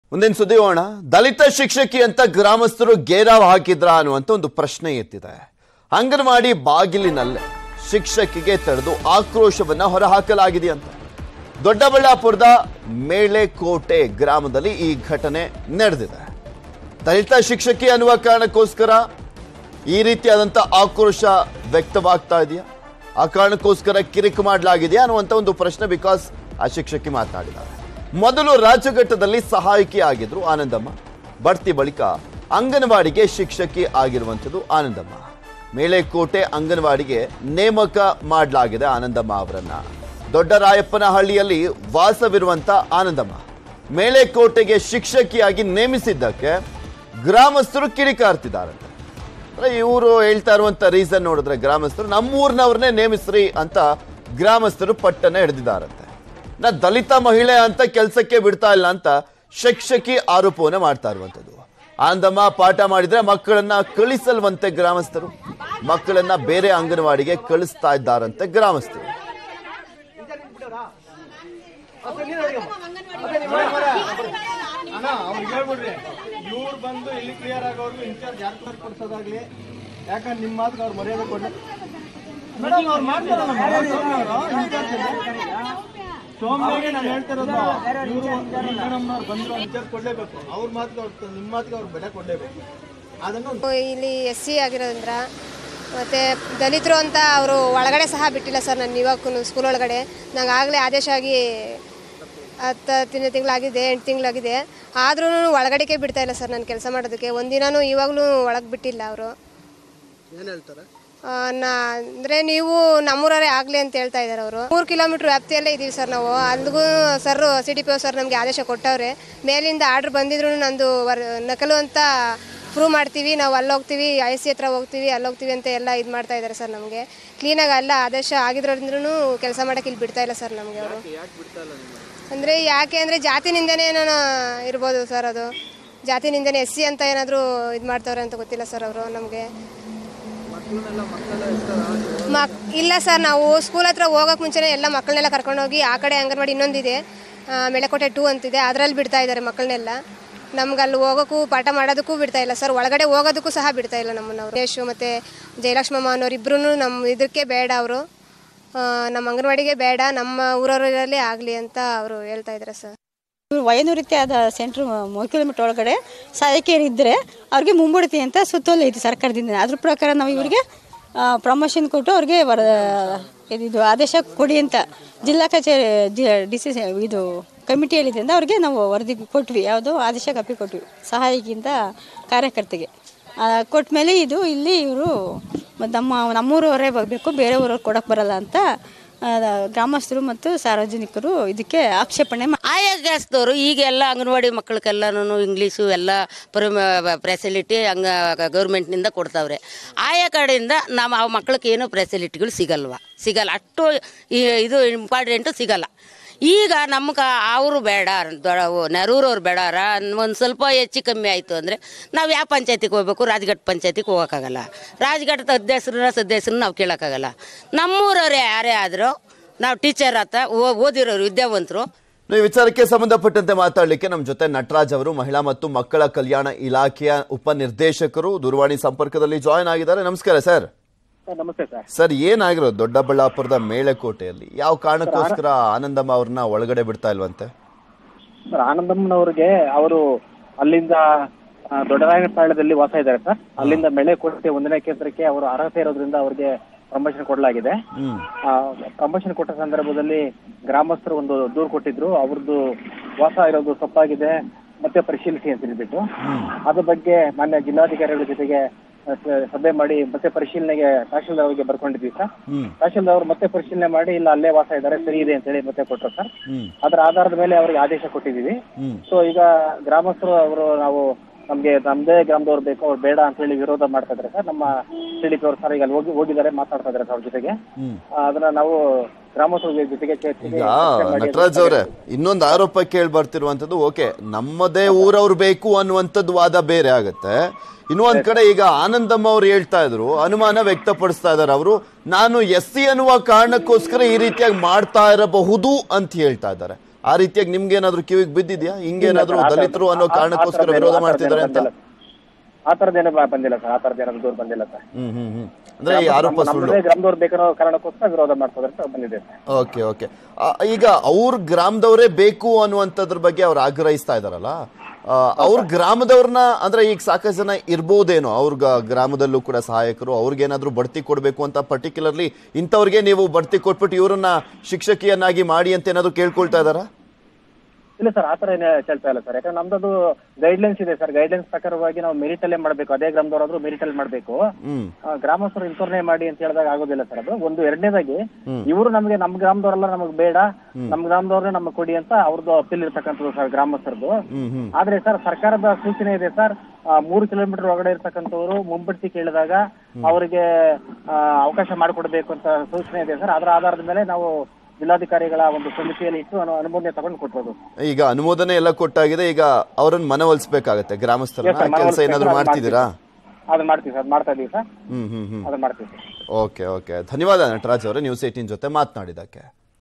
Doddaballapura Dalith Teacher Fight | ಅಂಗನವಾಡಿ ಬಾಗಿಲಿನಲ್ಲೇ ಶಿಕ್ಷಕಿಗೆ ತಡೆದು ಆಕ್ರೋಶ
Karnataka Dalith Teacher Fight |ನೇಮಕ‌ ಆಗಿ ಎರಡು ತಿಂಗಳು ಆದರೂ ಕೆಲಸ ಮಾಡಲು ಇನ್ನೂ ಬಿಟ್ಟಿಲ್ಲ. ದಲಿತ ಮಹಿಳೆ ಎಂದು ಕೆಲಸ ಮಾಡಲು ಹಿಡುತ್ತಿಲ್ಲ ಎಂದು ಶಿಕ್ಷಕಿ ಆರೋಪ ಮಾಡಿದ್ದಾರೆ.